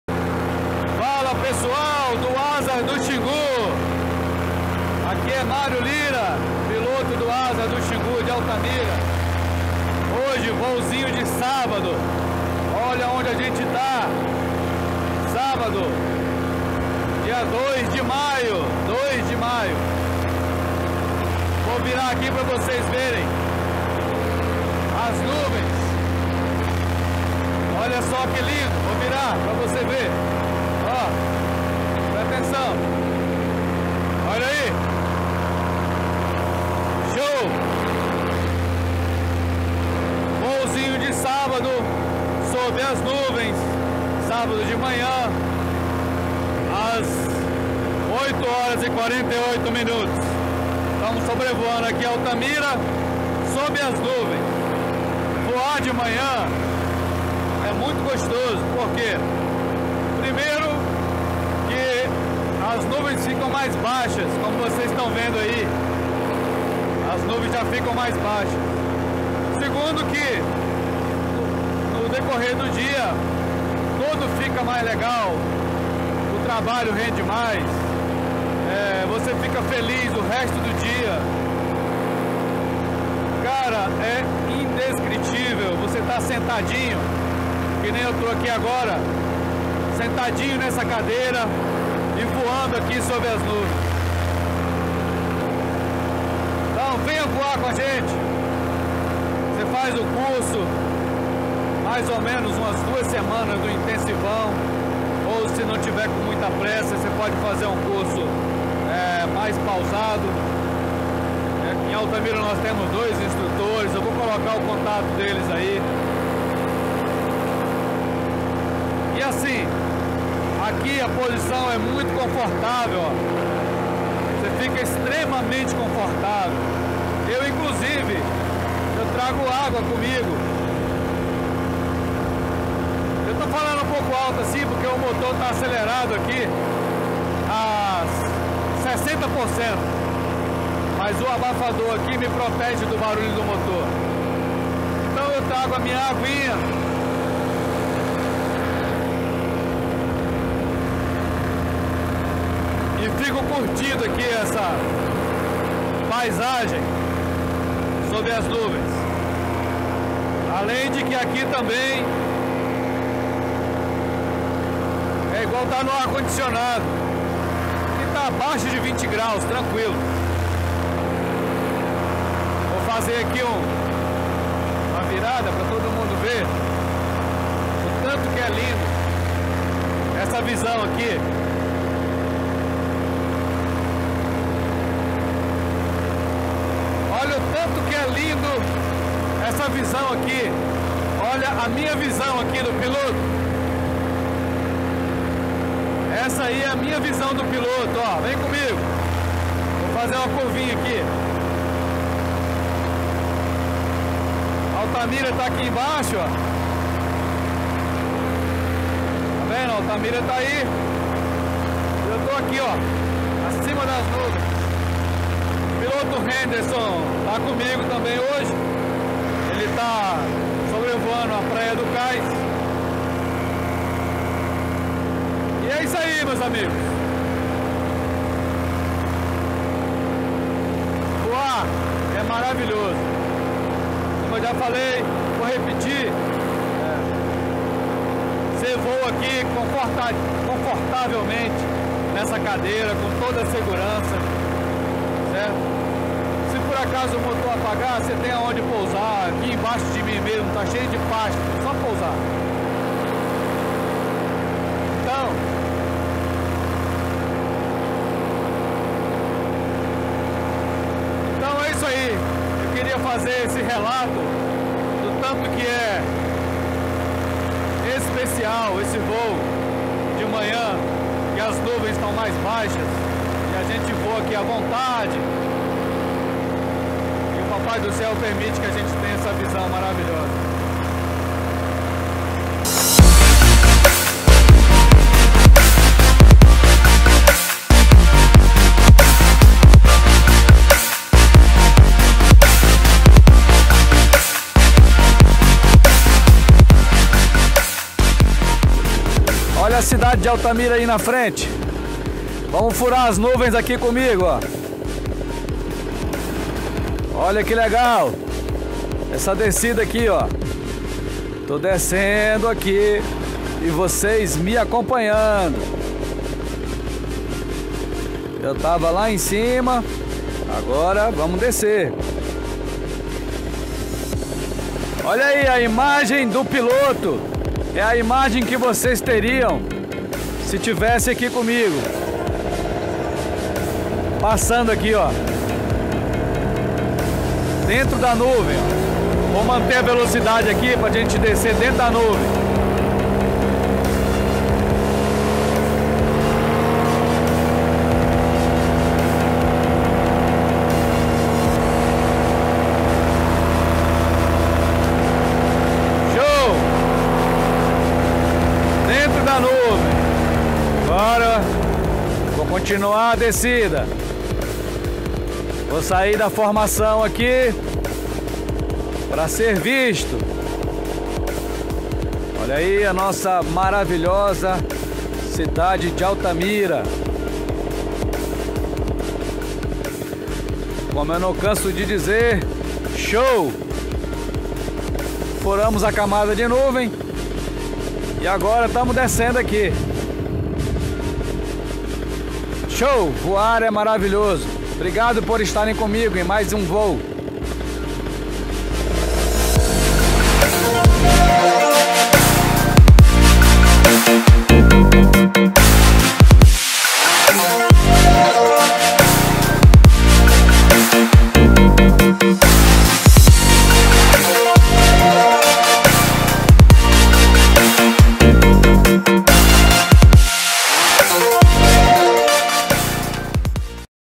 FALANDO DE PARAMOTOR NAS NUVENS
Piloto Mário Lira fala um pouco sobre o voo de paramotor nas nuvens.